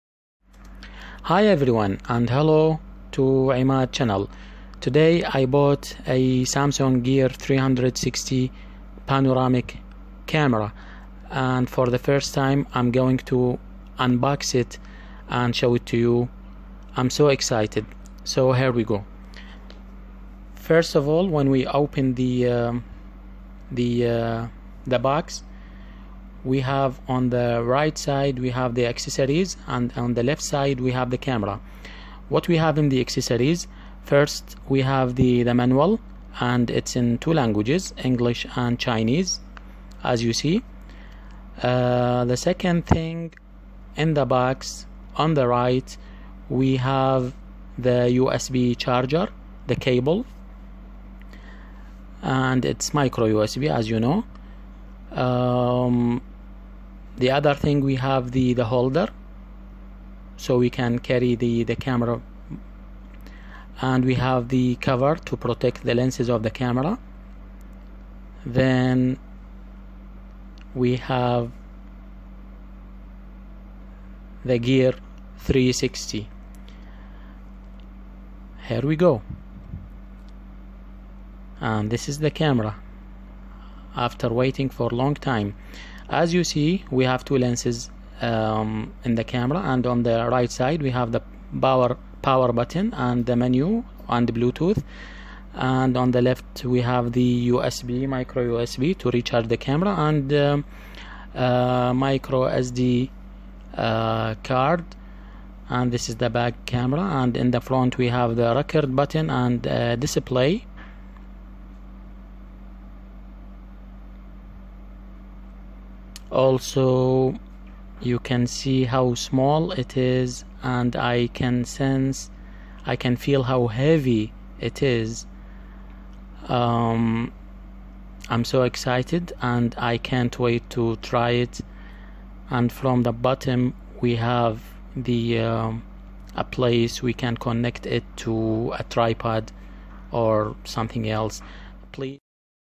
Samsung gear 360 2017 unboxing and review
My first time in 360 panoramic photos with Samsung gear